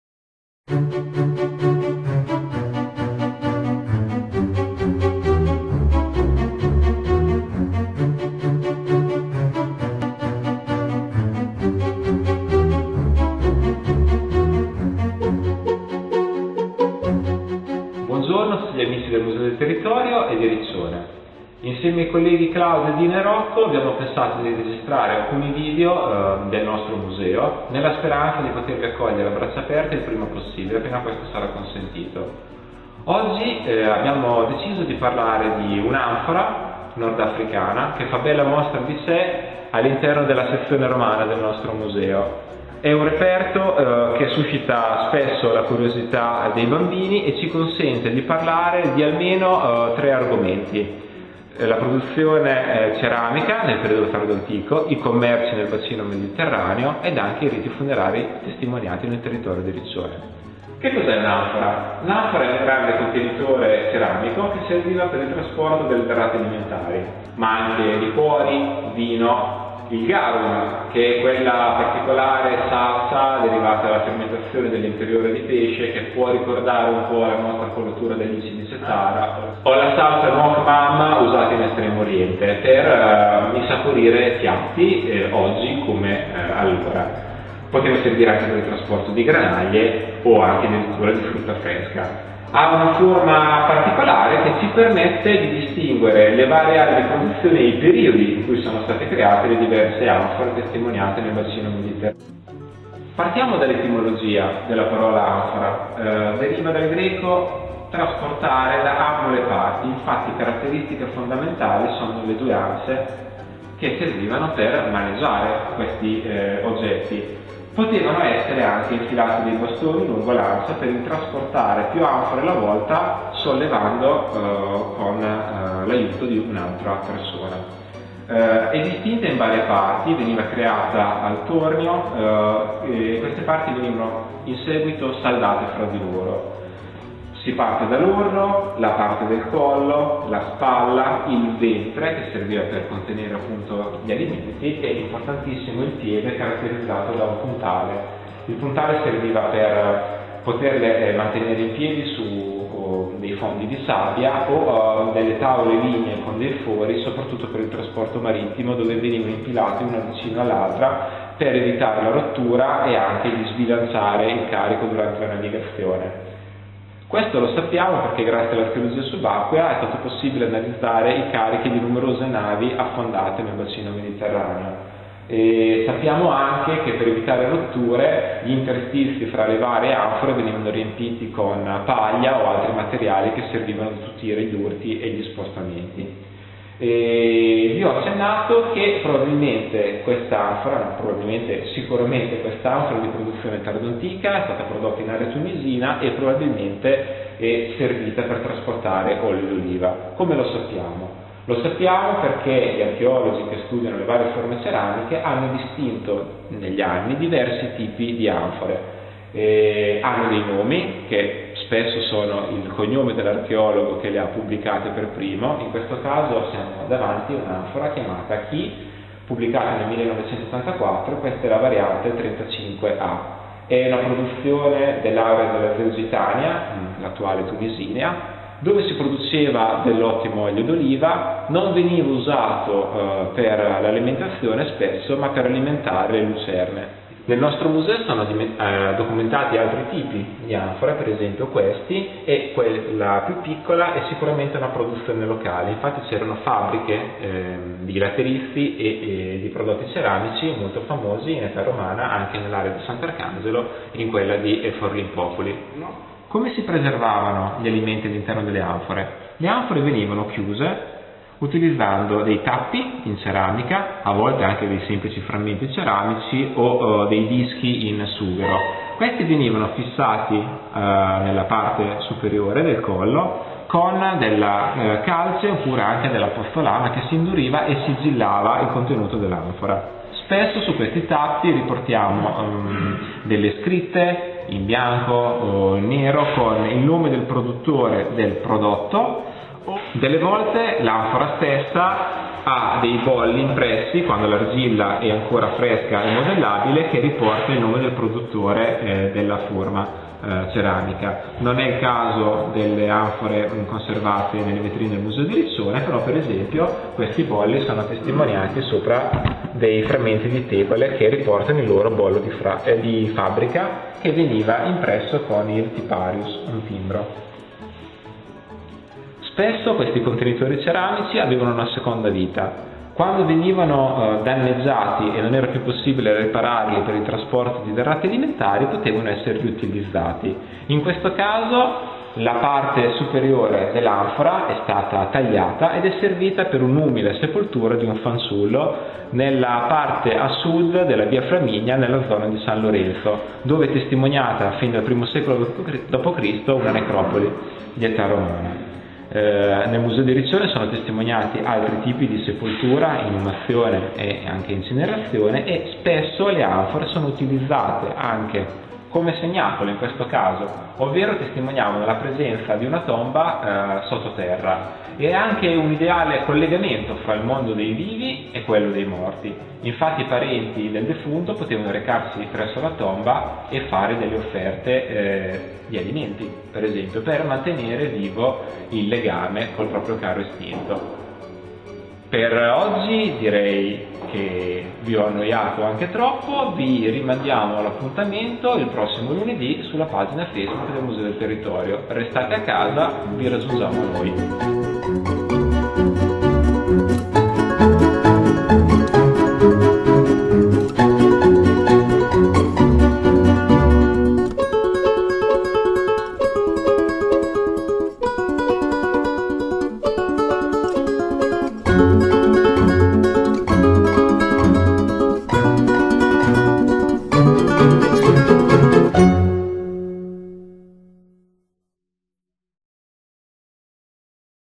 Museo del Territorio Riccione: Storia di un'anfora romana
Riccione, 27 aprile 2020. Visita guidata al Museo del Territorio di Riccione (parte 1).